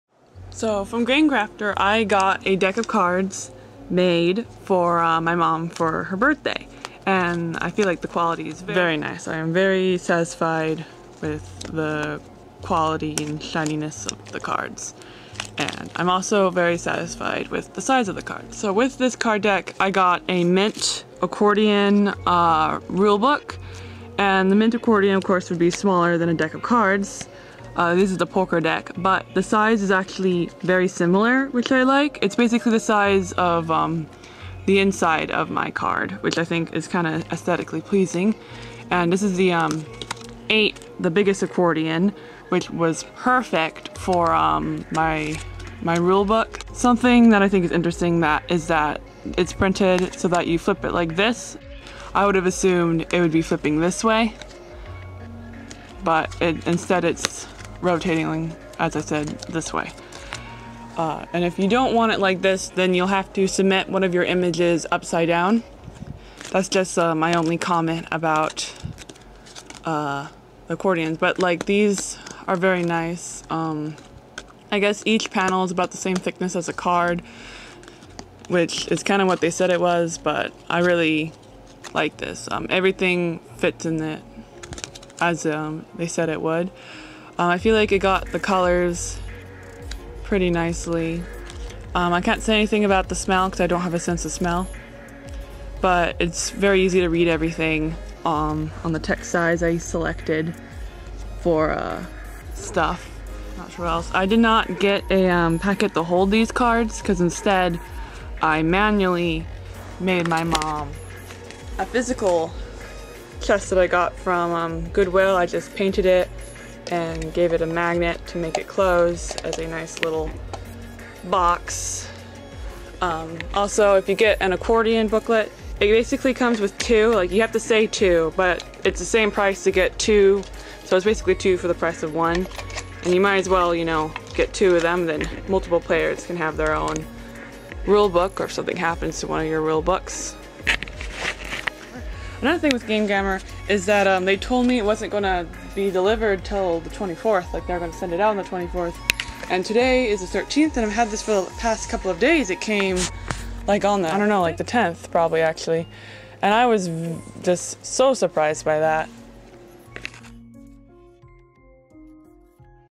Gamecrafter Review: Mint Tin Accordion (8 panels) and Poker Deck Cards
I ordered a Mint Tin Accordion (8 panels) and a deck of Poker size cards from theGamecrafter.com .
It's for a game I made called "Thieving Dogs (泥棒犬) which is just Exploding Kittens but with a new coat of paint. Being cute rather than gross.

The joke is that we call our dogs Kennel 'Jail' and say 'Go to jail' whenever we want her to go into it. Buuki, my mom's dog, is also a thief, and enjoys stealing things from the coffee table in our living room. Commonly stolen items include, but are not limited to; note cards, books, and pens. She steals stuff for the sake of stealing things, as, if we ignore her, she starts barking and showing off the stolen goods, prancing around with them, and dropping them right in front of us, and quickly grabbing them if we look at her.
Since I'm also learning Japanese, the game has Japanese text on it.
The 'family' cards, have my little sisters Pom and Huskey, my older sister's puppy, and Ipo, our family's old dog who passed away a few years ago.

For 56 cards and 2 mint rule books, the game cost 12, but it also was $6 to ship it and $1 tax, so it cost about 19 to get the game.
if anyone wants, I can send you one of the PDF images I put into game crafter if you're confused about text sizing or something